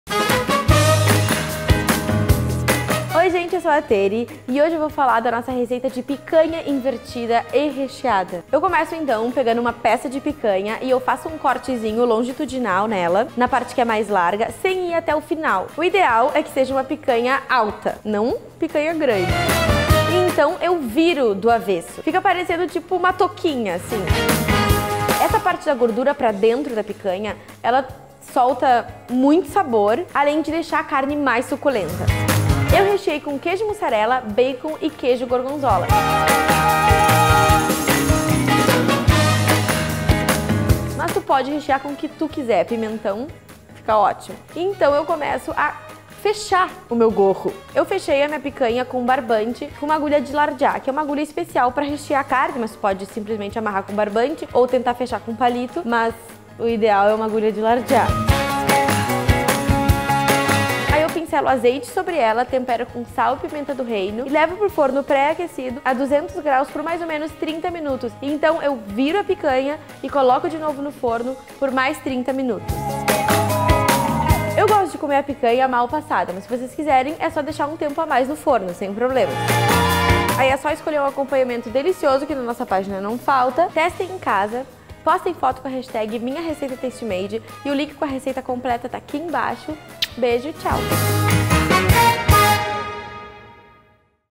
PICANHA INVERTIDA | Receitas Comentadas pela There
Essa receita de picanha invertida que a There ensina hoje fica super suculenta e gostosa!